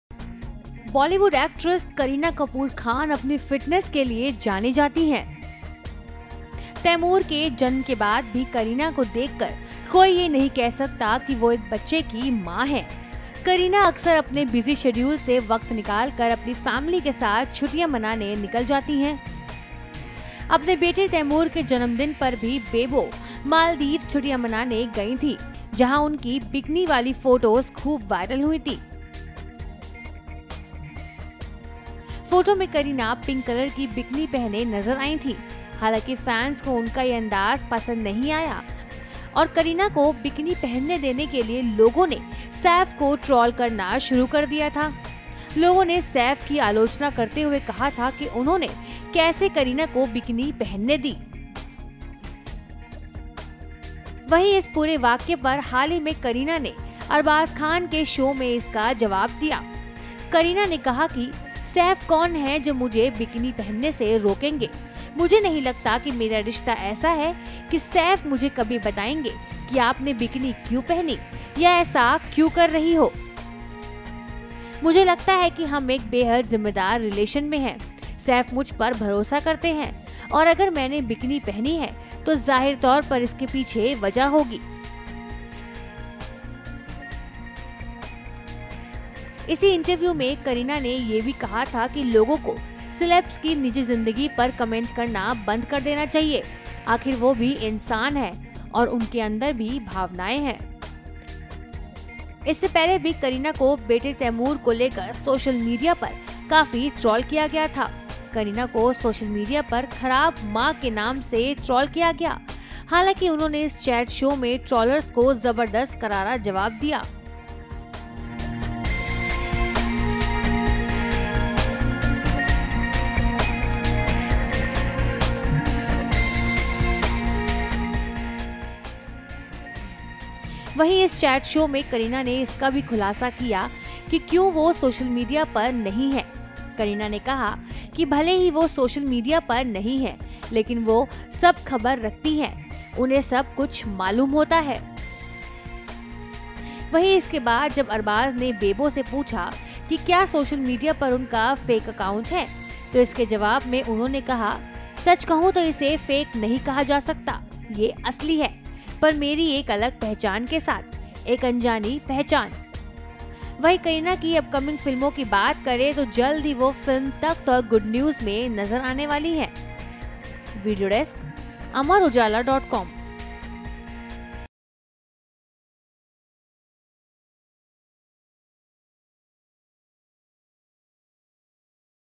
Kareena के bikini पहनने पर troll हुए थे Saif, अब Bebo ने कहा 'Saif कौन हैं जो मुझे bikini...'
Kareena Maldives holidays बिताने गई थीं। जहां उनकी Bikini Photos काफी viral हुई थीं। फोटो में Kareena pink color की bikini पहने नजर आई थीं। हालांकि fans को उनका ये अंदाज पसंद नहीं आया और Kareena को bikini पहनने देने के लिए लोगों ने Saif को troll कर दिया था।